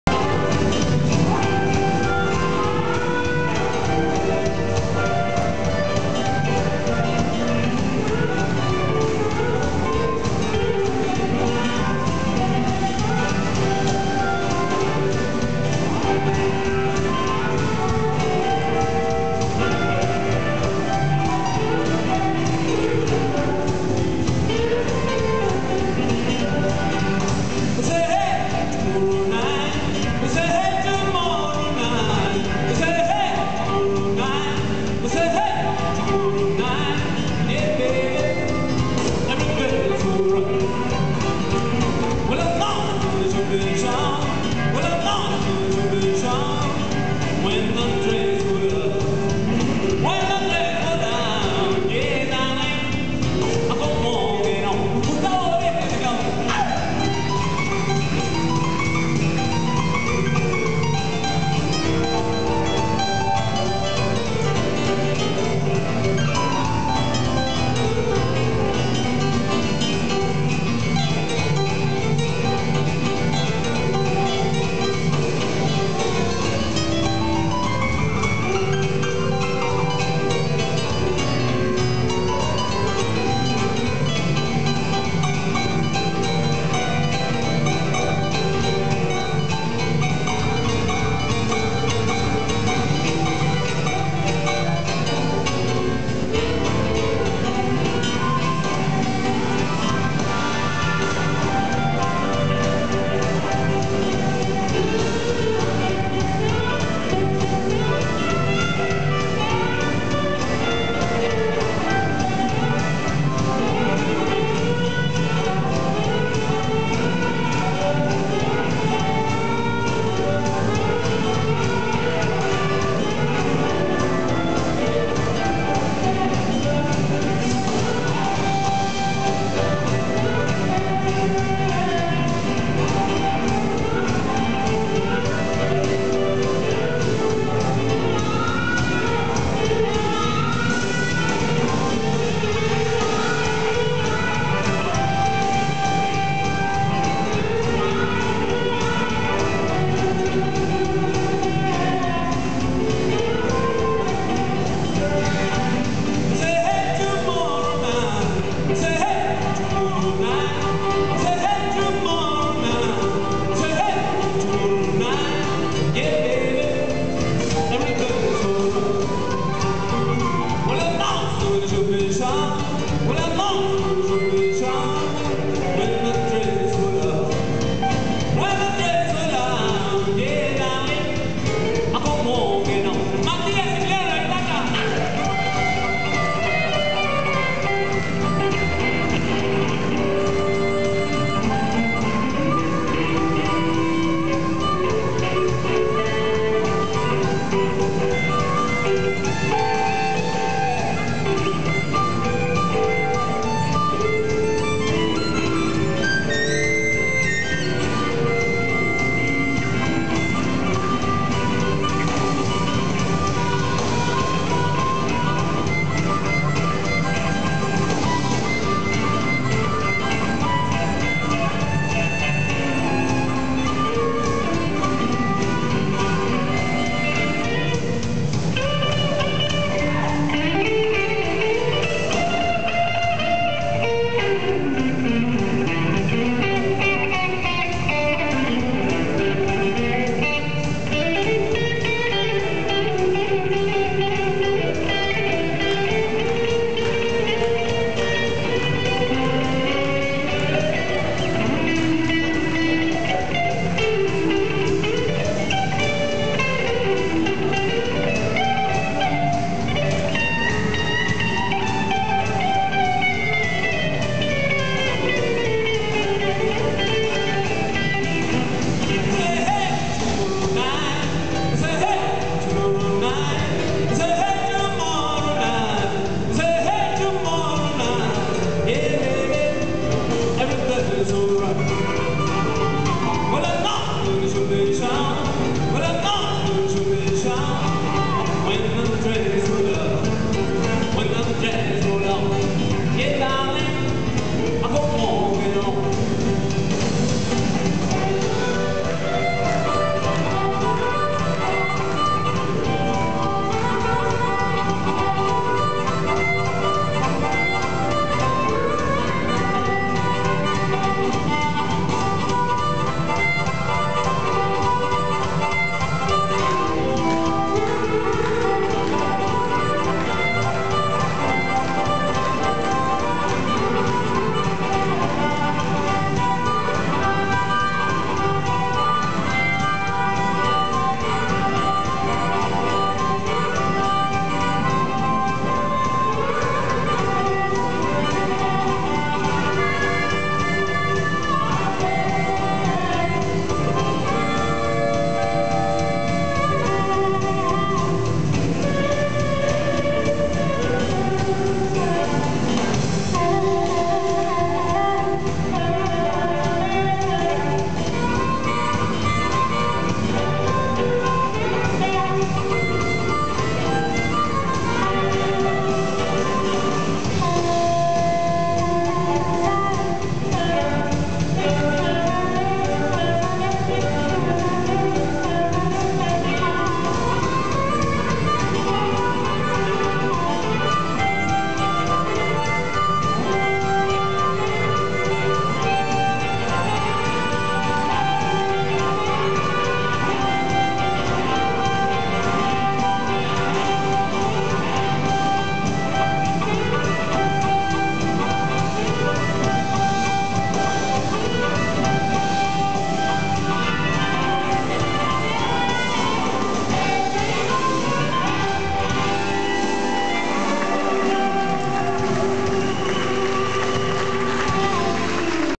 1er festival de Blues, La Plata Ciudad Blues, Fecha 2, Jefferson Goncalves & Shakedancers 2
1er festival blues "La Plata Ciudad Blues"
Fecha 2, Viernes 15 de febrero del 2013
Teatro Bar, La Plata
Jefferson Goncalves & Shakedancers
Gondwana Producciones S.R.L.